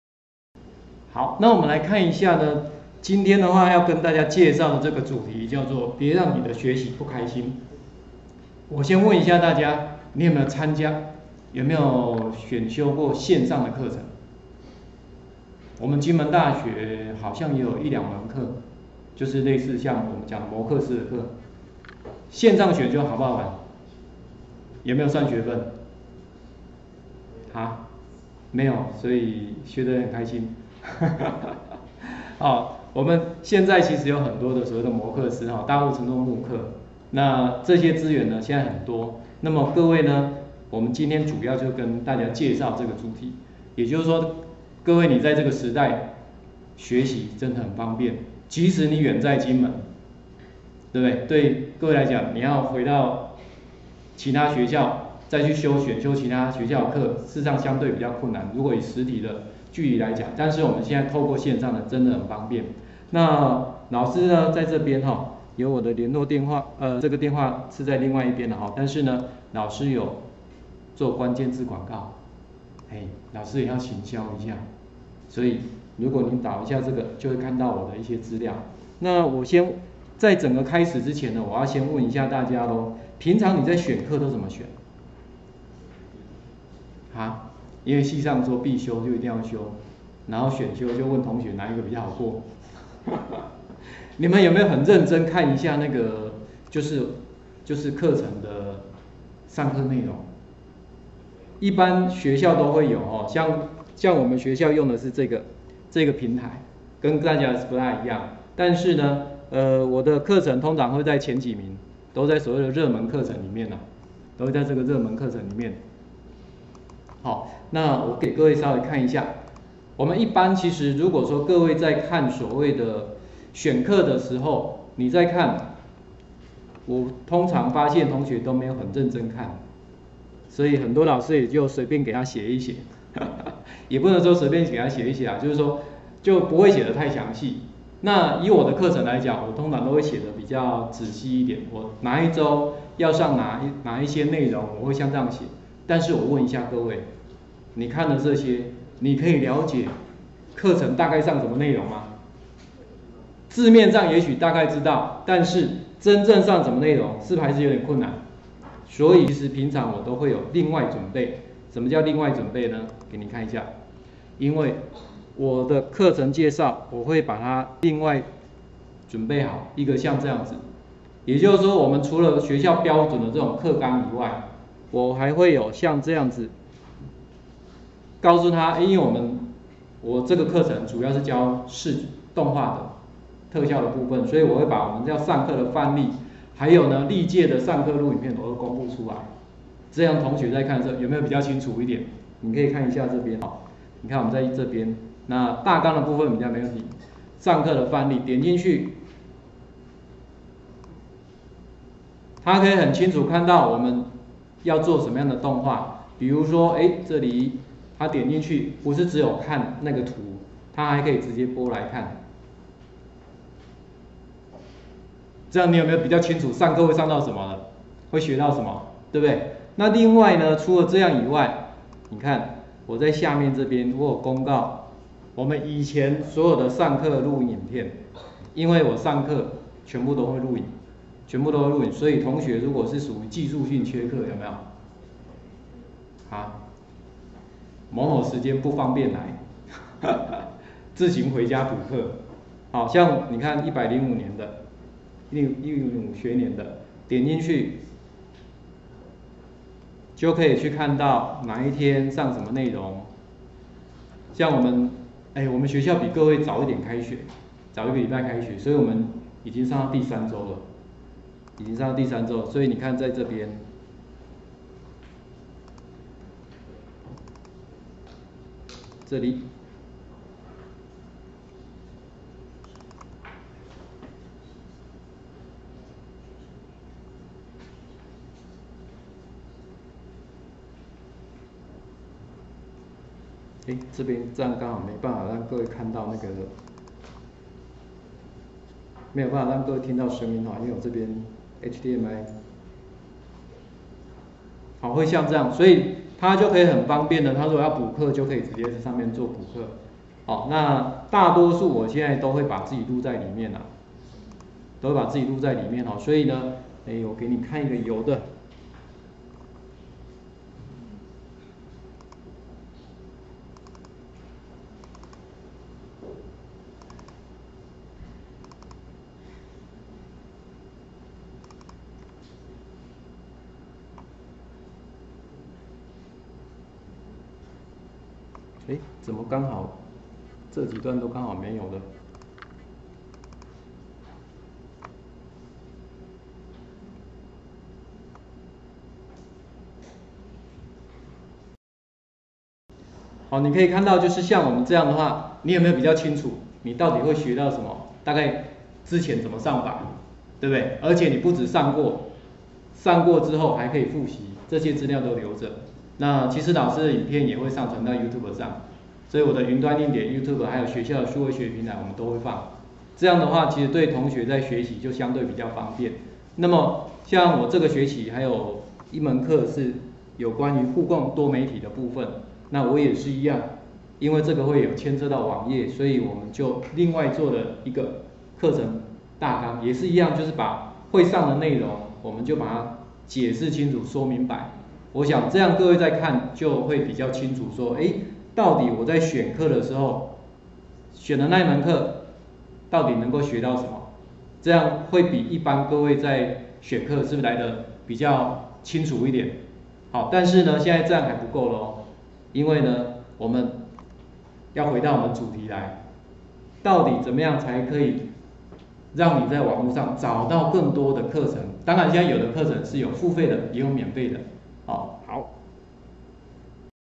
02：在大學四年中，您都怎麼選課？述文老師動態視覺與多媒體互動課程介紹
此研習錄影影片為「20170301金門大學：我的雲端數位時代1.0：別讓你的學習不開心，一切就從開放課程+自主學習開始」的研習錄影！

想要跨領域學習嗎？看到有興趣的課程，也想一起來學習嗎？別讓你的學習不開心，一切就從開放課程+自主學習開始！國外有 Coursera、Udacity、edX、Marginal Revolution University，臺灣也有 ewant育網、Share Course、TaiwanLIFE…等，眾多課程就等你一起來看看！

MOOCs 課程在台灣稱為「磨課師」，大陸稱做「慕課」，Massive Open Online Courses, MOOCs）是近幾年國內外大學興起的大規模免費線上開放式課程，以小單元的分段影片課程，在配合線上討論與回饋、線上同儕合作學習與討論、虛擬線上實驗及線上練習與評量的課程，學生可以依自己學習的速度安排學習進度。